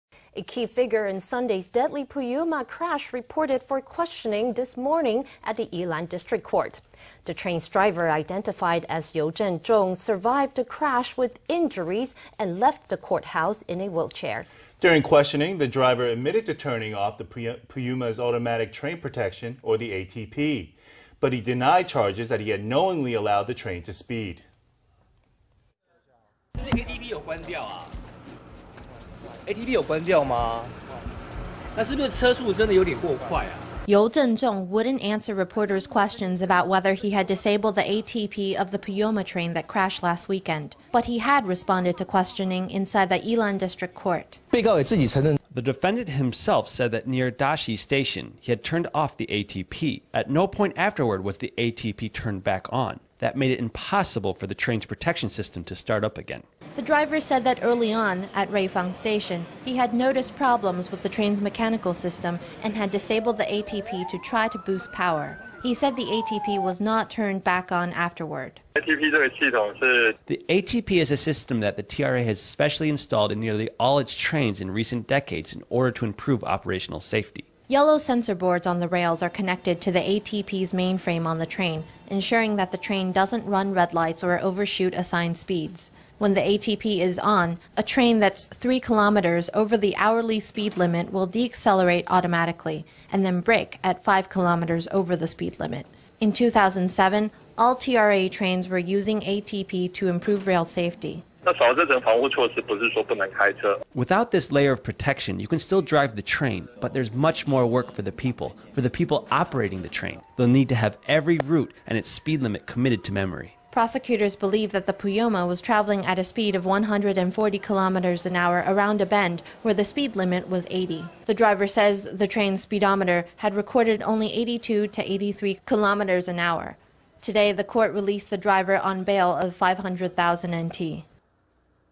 Puyuma driver says he disabled ATP but didn’t speed intentionally
A key figure in Sunday’s deadly Puyuma crash reported for questioning this morning at the Yilan District Court. The train’s driver, identified as Yu Chen-chung, survived the crash with injuries and left the courthouse in a wheelchair. During questioning, the driver admitted to turning off the Puyuma’s automatic train protection, or the ATP. But he denied charges that he had knowingly allowed the train to speed.Yu Chen-chung wouldn’t answer reporters’ questions about whether he had disabled the ATP of the Puyuma train that crashed last weekend. But he had responded to questioning inside the Yilan District Court. Huang Yong-shengYilan District Court judgeThe defendant himself said that near Daxi station, he had turned off the ATP. At no point afterward was the ATP turned back on. That made it impossible for the train’s protection system to start up again.The driver said that early on, at Ruifang station, he had noticed problems with the train’s mechanical system and had disabled the ATP to try to boost power. He said the ATP was not turned back on afterward.Voice of Ren Hen-yiSociety of Railway and National PlanningThe ATP is a system that the TRA has specially installed in nearly all its trains in recent decades in order to improve operational safety.Yellow sensor boards on the rails are connected to the ATP mainframe on the train, ensuring that the train doesn’t run red lights or overshoot assigned speeds. When the ATP is on, a train that’s 3 kilometers over the hourly speed limit will decelerate automatically, and then brake at 5 kilometers over the limit. In 2007, all TRA trains were using ATP to improve rail safety.Voice of Ren Hen-yi Society of Railway and National PlanningWithout this layer of protection, you can still drive the train, but there’s much more work for the people, for the people operating the train. They’ll need to have every route and its speed limit committed to memory. Prosecutors believe the Puyuma was traveling at a speed of 140 kilometers an hour around a bend where the speed limit was 80. The driver says the train’s speedometer had recorded only 82 to 83 kilometers an hour. Today the court released the driver on bail of NT$500,000.